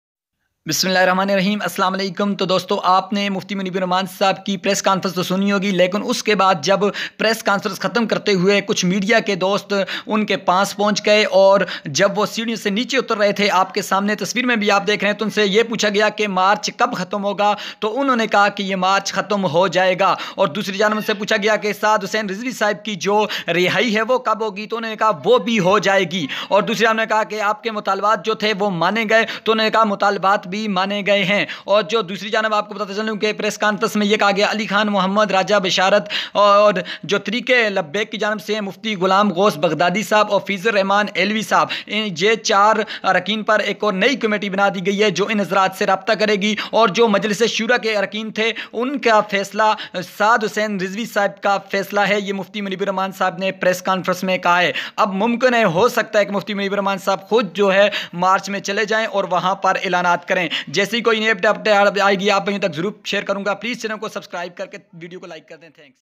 پریس کانفرنس کے بعد مفتی صاحب نے سعد حسین رضوی رہائی مارچ ختم کا اعلان کیا long march detail
Dalola Official